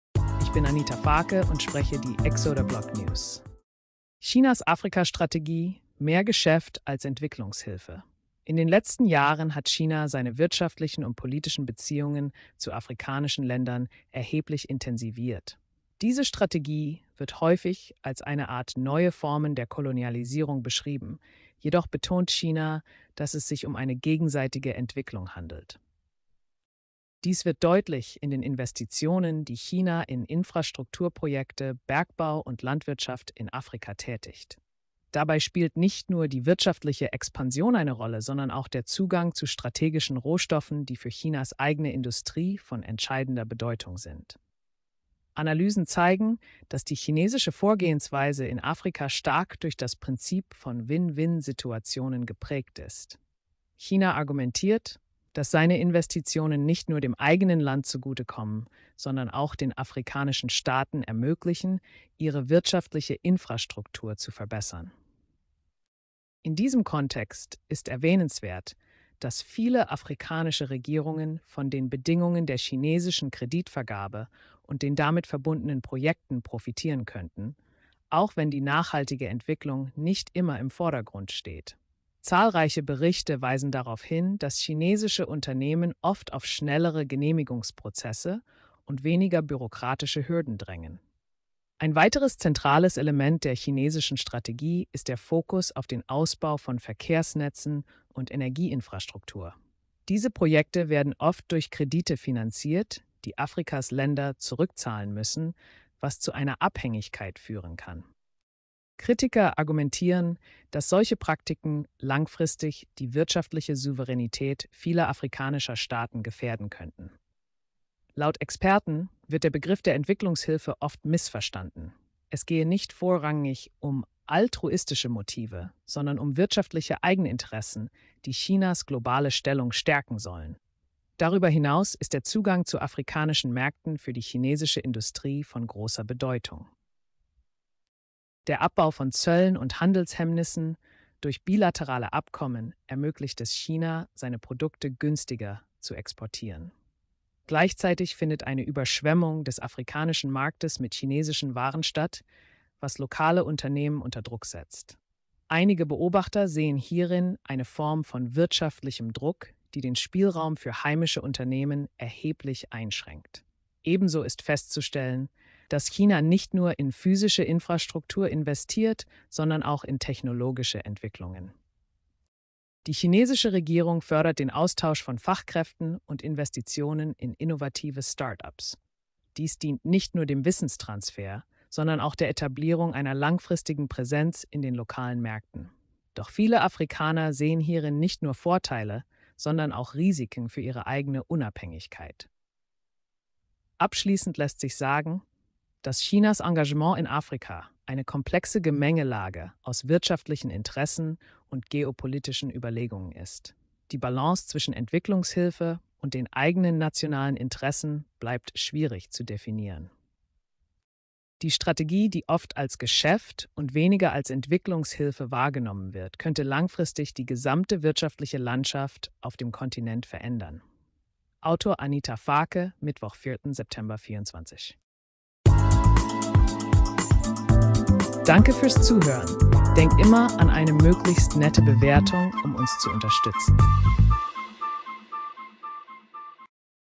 Chinas Afrika-Strategie: Wirtschaftliche Interessen ueber Entwicklungshilfe (Podcast)
Chinas Strategie in Afrika fokussiert sich auf wirtschaftliche Beziehungen, die oft als neue Form der Kolonialisierung kritisiert werden. China investiert stark in Infrastruktur, Bergbau und Landwirtschaft, um Zugang zu wichtigen Rohstoffen zu gewinnen. Diese Investitionen sollen als Win-Win-Situationen fungieren, wobei die afrikanischen Laender von verbesserten Infrastrukturen profitieren koennten, ohne dass nachhaltige Entwicklung immer im Vordergrund steht. Kritiker warnen vor einer Abhaengigkeit durch Kreditvergabe, die die Souveraenitaet der afrikanischen Staaten gefaehrden koennte. Zudem draengt China auf den Abbau von Handelshemmnissen, was lokale Unternehmen unter Druck setzt. In Summe zeigt sich eine komplexe Gemengelage aus wirtschaftlichen Interessen und geopolitischen Ambitionen. [-1] AI-generated content